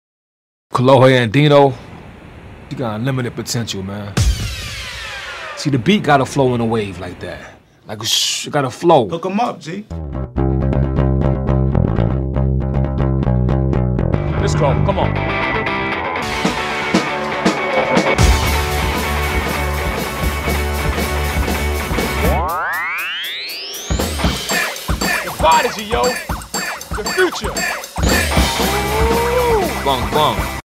Nike 6.0 Commercial - The RZA remixes for Kolohe Andino
In the latest Nike 6.0 commercial series, the brand brought on board The RZA. Check out the video here above where he remixes for Kolohe Andino.